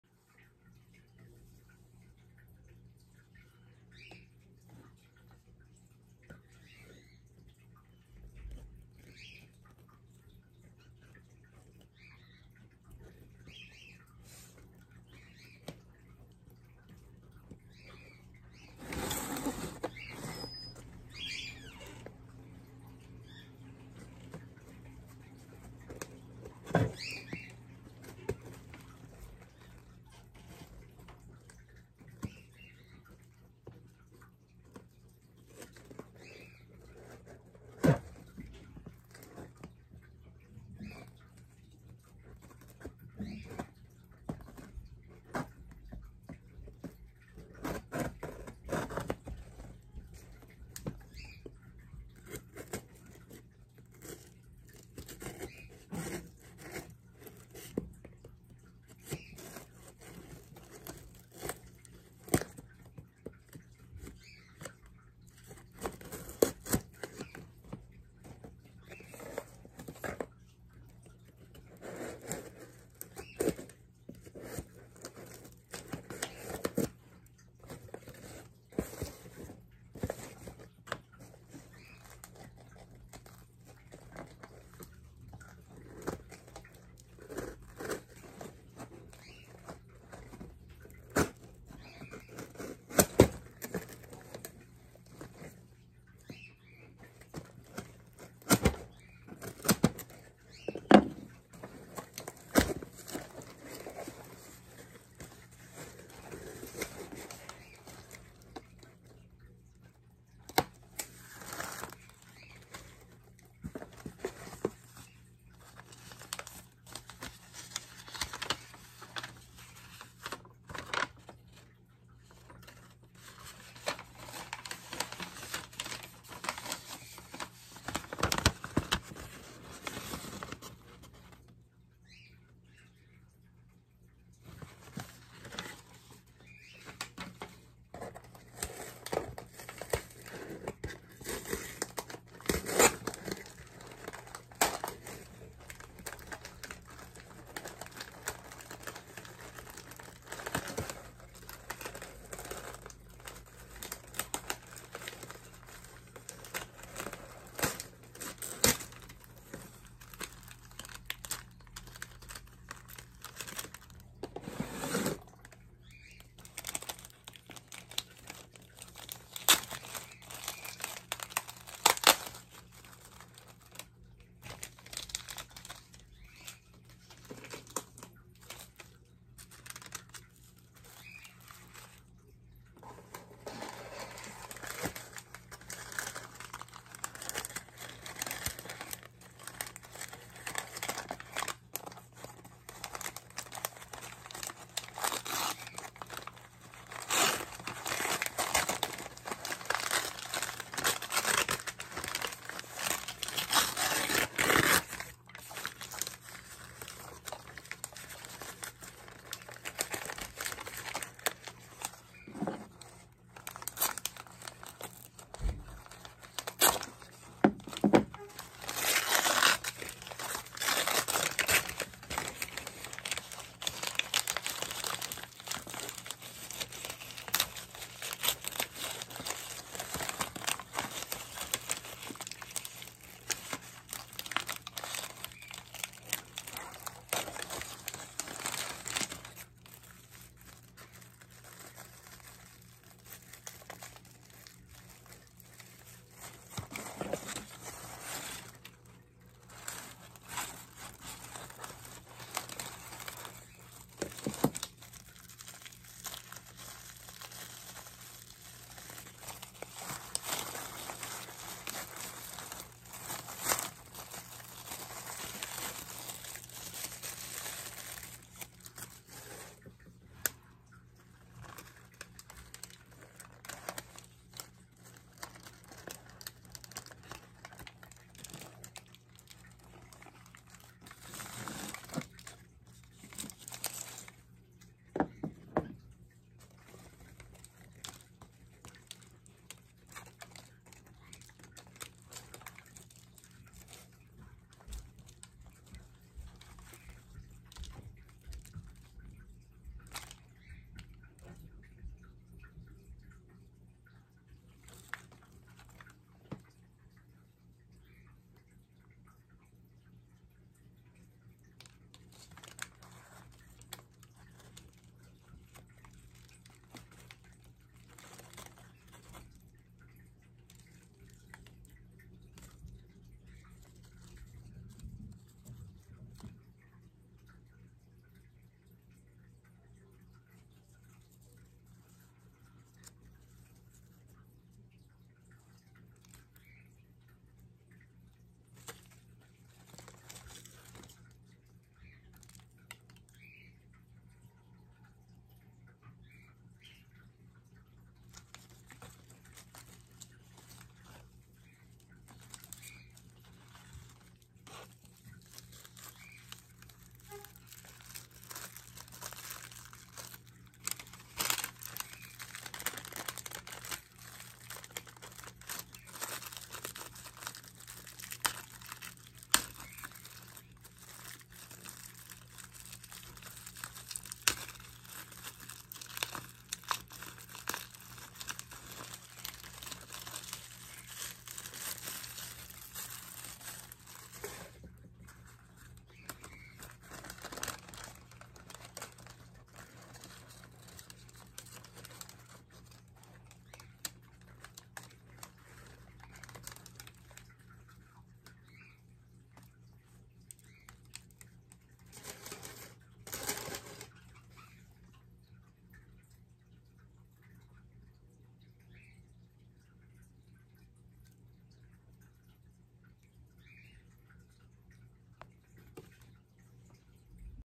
Unboxing syngoniums from Etsy
I bought a pack of three different syngoniums. They also sent me a freebie of a holiday cacti. Awesome lady to work with and is packaged really well. The plants are tiny, but extremely beautiful and cute. I’ll link her Etsy shop tropicalplantlady.